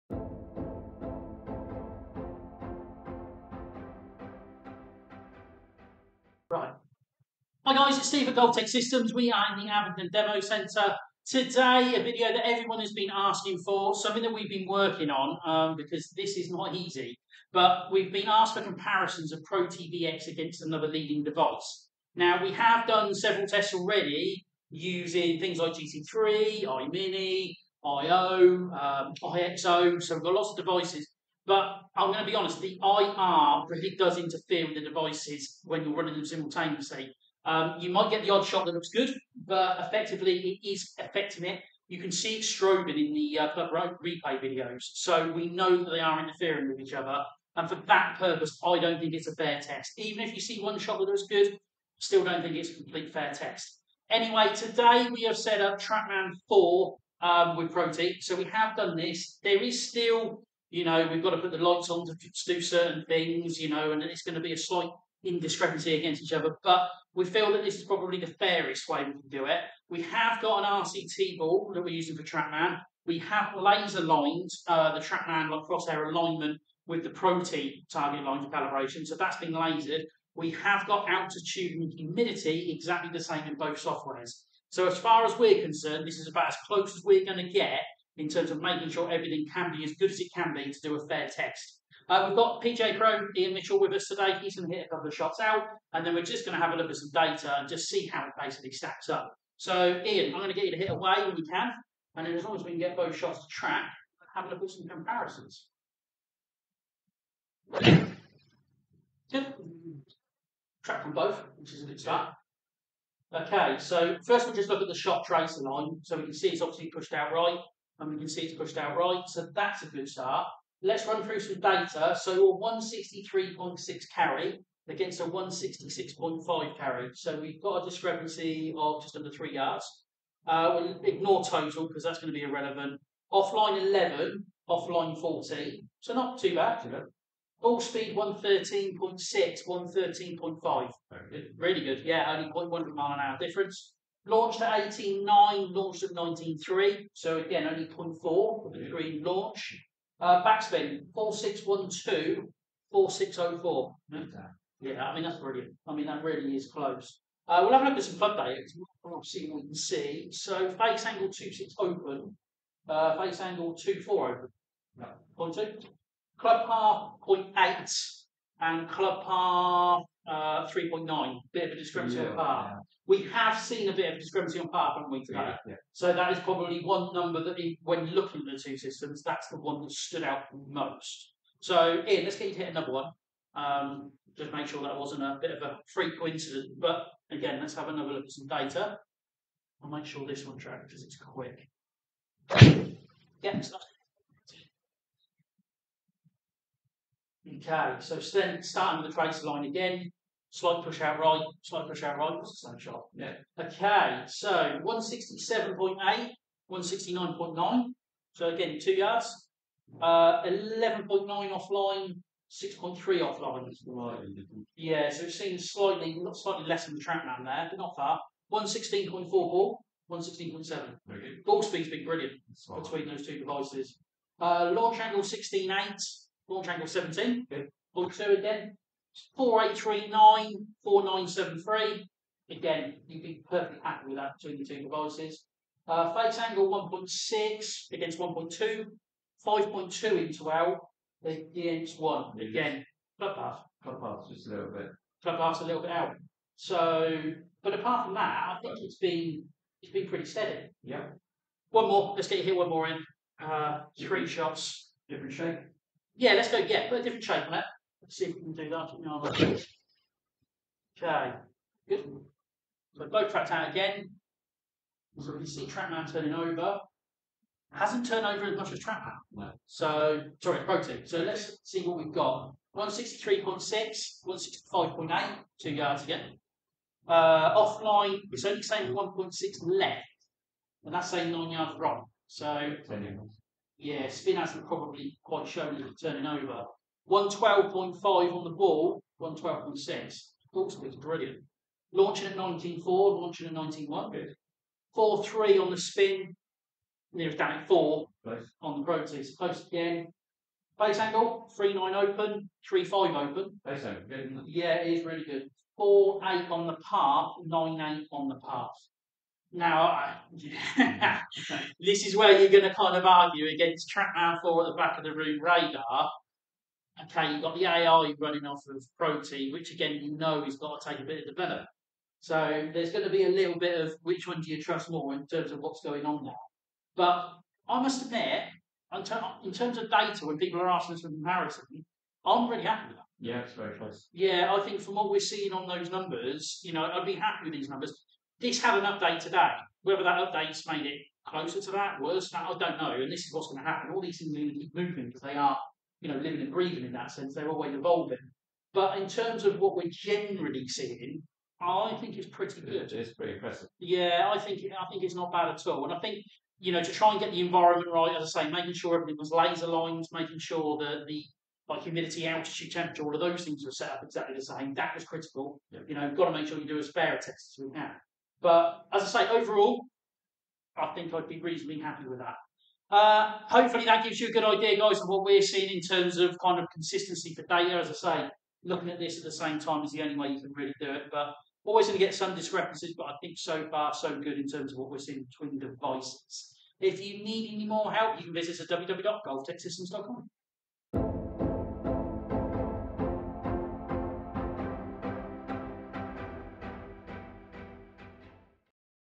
ProTee VX Compared to Trackman 4
Comparing a few shots between the ProTee VX and Trackman 4. Systems have been laser line calibrated and altitude and temperature set to the same in both softwares